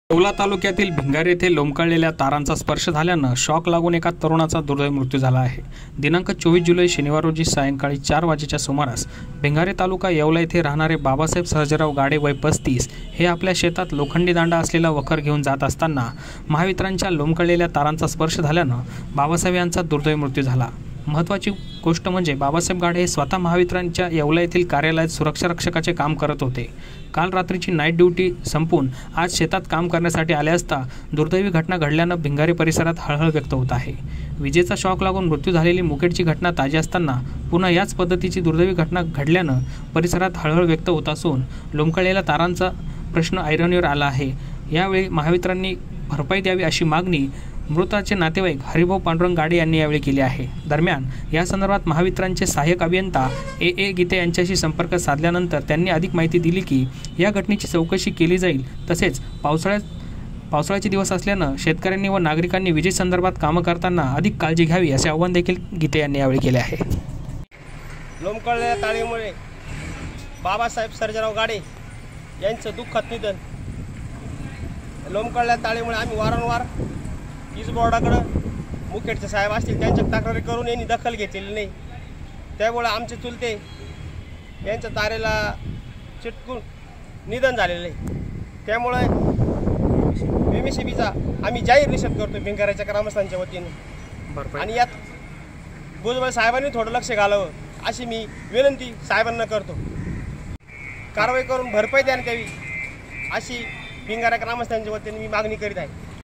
येवला :-"भिंगारे येथे लोंबकळलेल्या तारांचा स्पर्श झाल्याने शॉक लागून तरुणाचा दुर्दैवी मृत्यू...!!
भिंगारे येथे लोंबकळलेल्या तारांचा स्पर्श झाल्याने शॉक लागून तरुणाचा दुर्दैवी मृत्यू

दिनांक 24 जुलै शनिवार रोजी सायंकाळी चार वाजेच्या सुमारास
भिंगारे तालुका येवला येथे राहणारे बाबसाहेब सर्जेराव गाडे वय 35 हे आपल्या शेतातून लोखंडी दांडा असलेला वखर घेऊन जात असताना महावितरणच्या लोंमकलेल्ल्या तारांचा स्पर्श झाल्याने  बाबासाहेब यांचा दुर्दैवी मृत्यू झाला
महत्त्वाची गोष्ट म्हणजे बाबसाहेब गाडे हे स्वतः महावितरणच्या येवला येथील कार्यालयात सुरक्षा रक्षकाचे काम करत होते
काल रात्रीची नाईट ड्युटी करून आज शेतात काम करण्यासाठी ते आले असता ही घटना घडल्याने भिंगारे परिसरात हळहळ व्यक्त होत आहे
विजेचा शॉक लागून मृत्यू झालेली मुखेड ची घटना ताजी असताना पुन्हा याच दुर्दैवी घटनेची पुनरावृत्ती झाली असून लोंबलेल्या तारामुळेच सदर घटना झाली असून महावितरणने याची भरपाई करावी अशी मागणी मृतांचे नातेवाईक हरिभाऊ पांडुरंग गाडे यांनी केली आहे
दरम्यान या संदर्भात महावितरणचे साहाय्यक अभियंता a a गीते यांच्याशी संपर्क साधल्यानंतर अधिक माहिती घेतली असता या घटनेची चौकशी केली जाईल मात्र सध्या पावसाळ्याचे दिवस असून शेतकऱ्यांनी व नागरिकांनी अधिक काळजी घेऊन विजेच्या संदर्भातील काम करावी असे आव्हान त्यांनी केले आहे